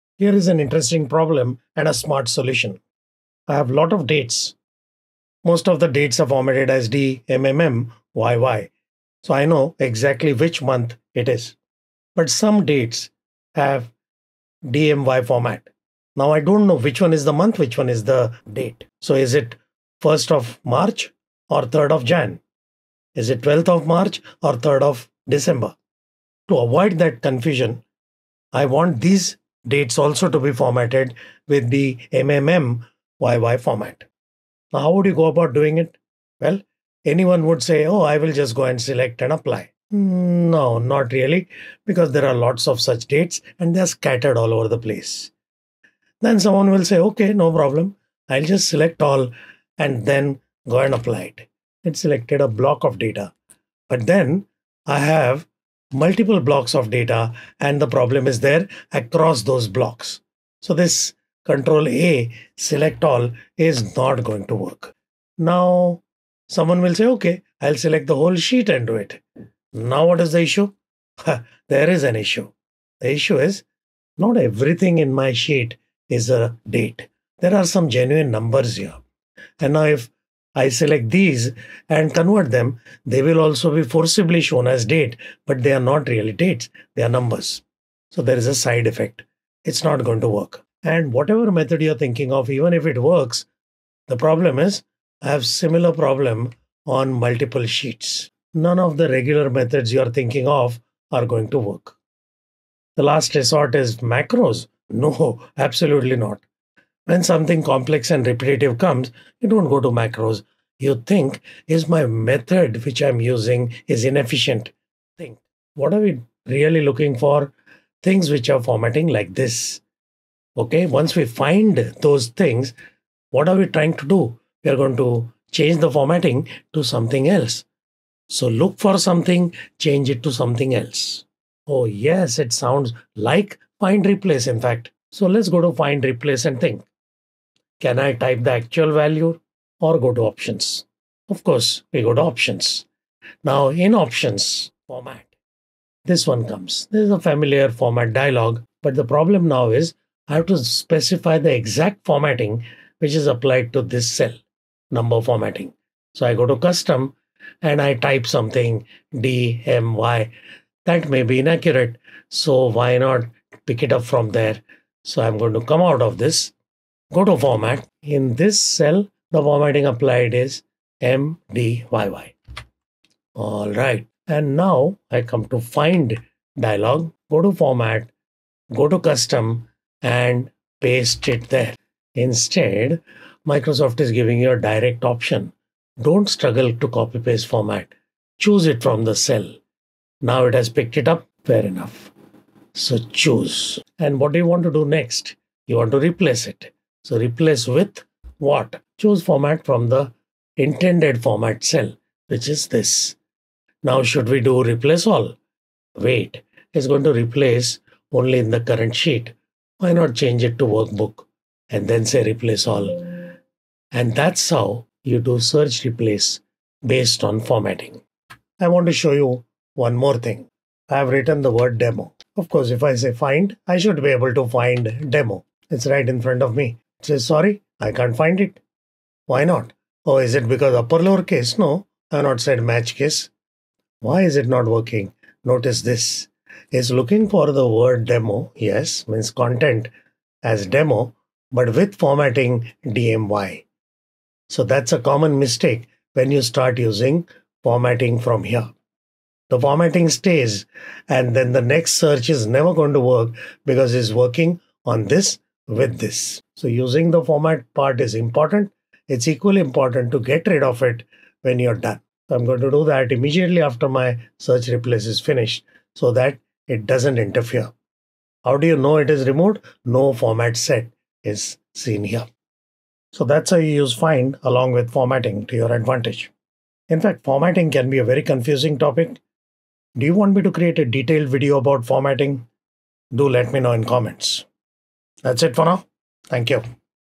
Excel - Find Replace Formatting
Learn a smart and useful technique to use Find Replace in Excel.

Contents
00:00 - The problem
00:34 - Obvious (but wrong) solutions
01:56 - The right solution
03:38 - Clear formatting

Efficiency Best Practices for Microsoft 365
130 best practices everyone must know